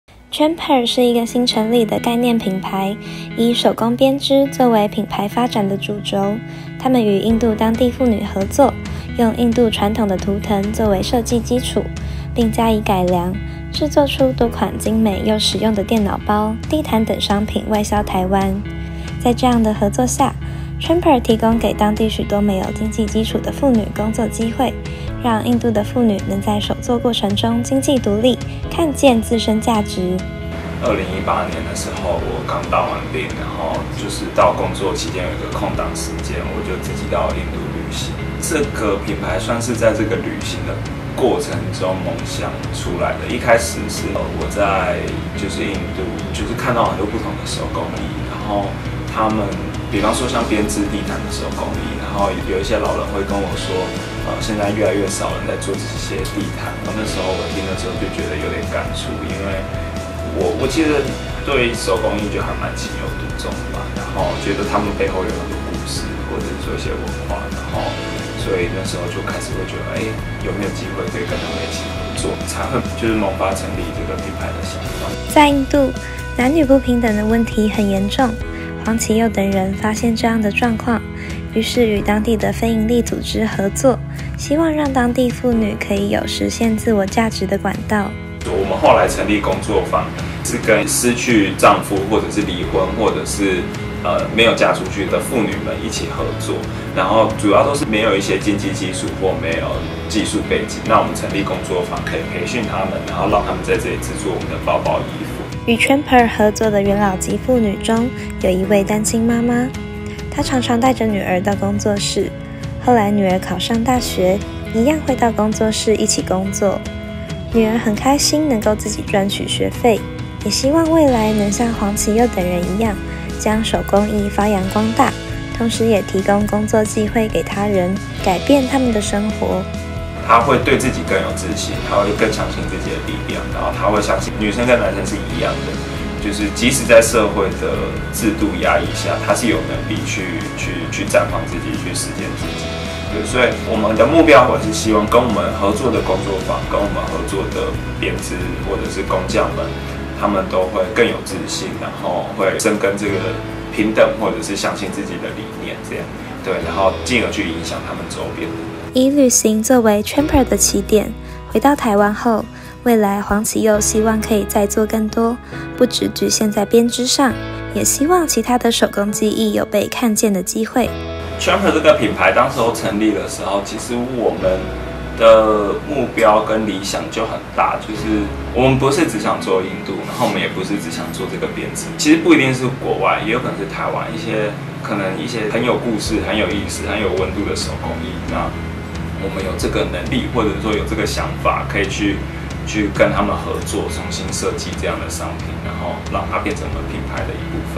台北｜公民記者｜Tramper手工藝品店 串聯台灣與印度
【中嘉台北 校園記者陳安蓉、黃雅詩／台北市中山區報導】
2018年，剛當完兵的黃啟祐獨自來到印度旅行，途中看到許多販賣各式手工藝品的攤商，印度是古代南方絲綢之路的必經之地，其製作地毯的工藝更是名聞遐邇，這些手工編織品主要都是由婦女來製作，但女性在印度的社會地位卻是相當低落的。黃啟祐希望她們都能找到自己的價值，因而萌發了與當地合作的想法，於隔年建立起印度婦女工作坊，並成立Tramper這個品牌，在台灣販售她們製作的包包、衣服等手工藝品，至今已培訓超過20名婦女，且持續給予婦女們優於當地行情的工資，幫助她們改善生活。